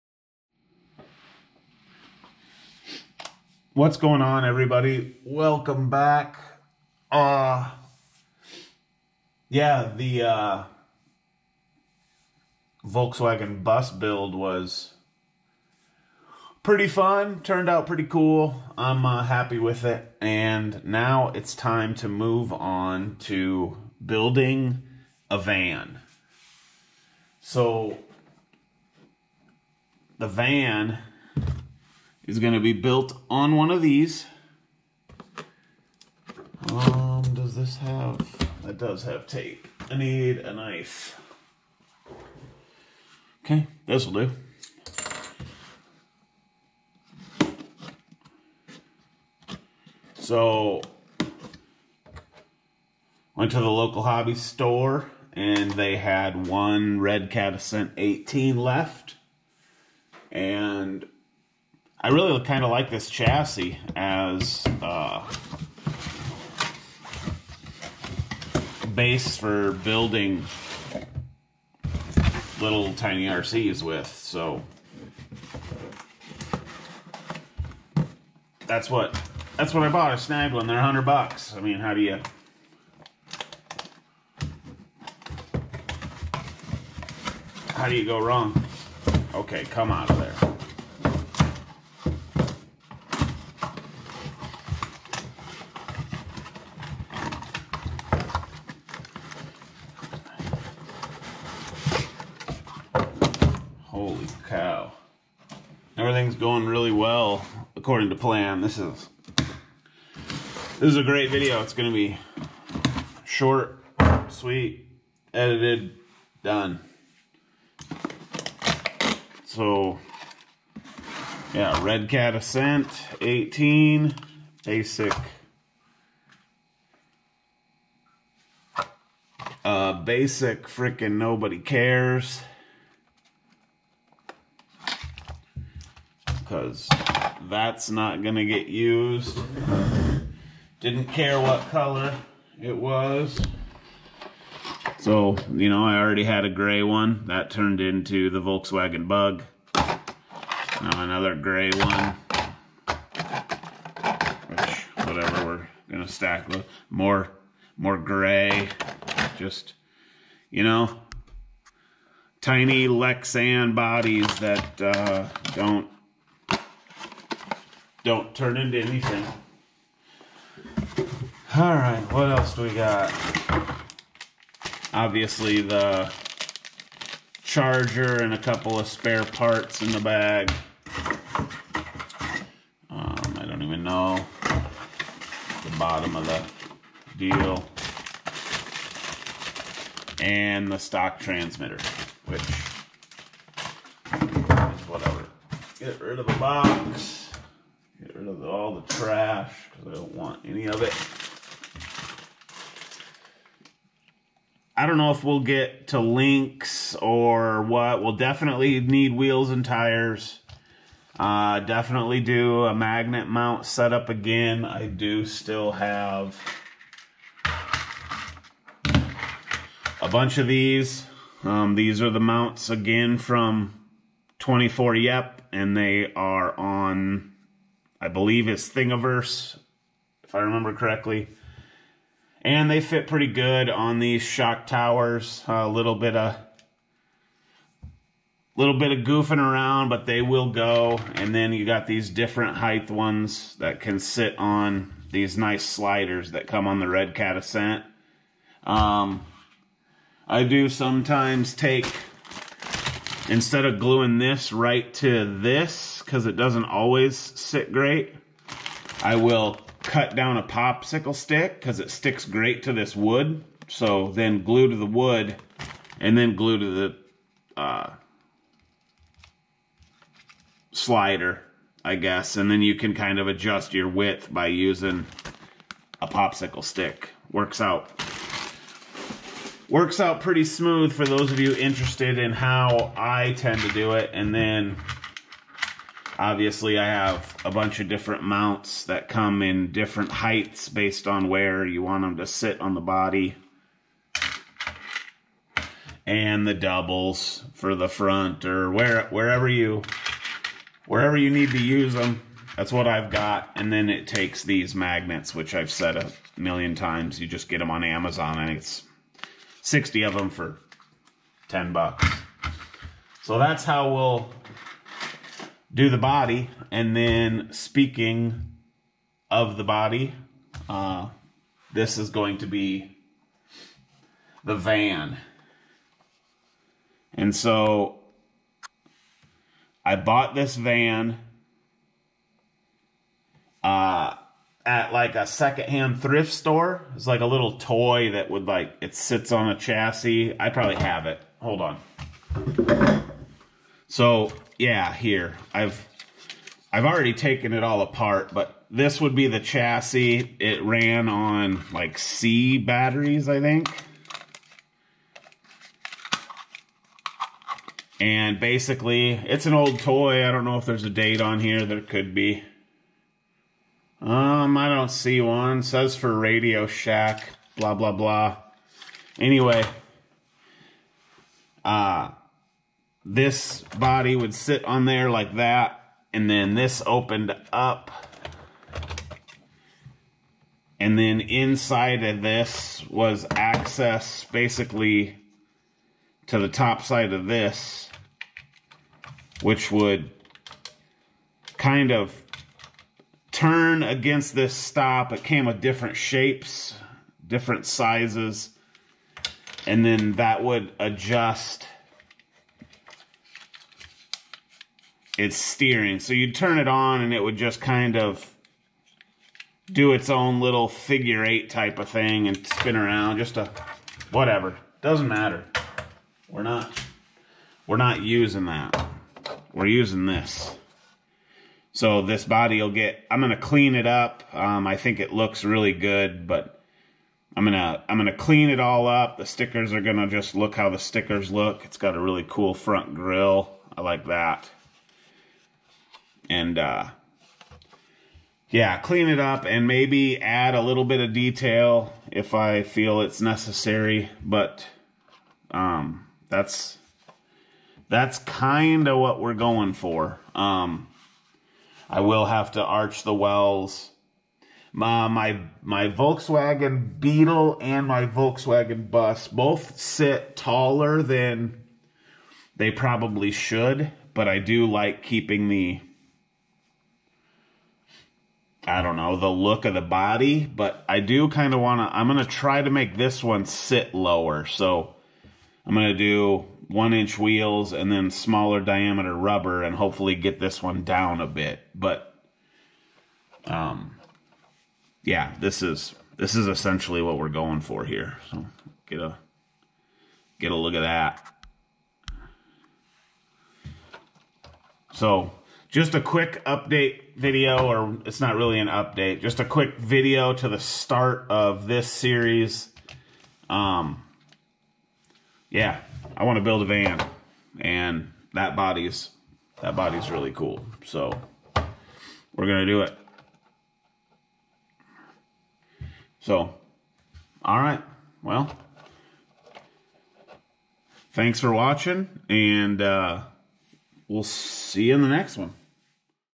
Mini RC van, part 1
Ive had this toy van for awhile.
Probably time to convert it to RC.
Snagged a Redcat Ascent 18.
Should do nicely.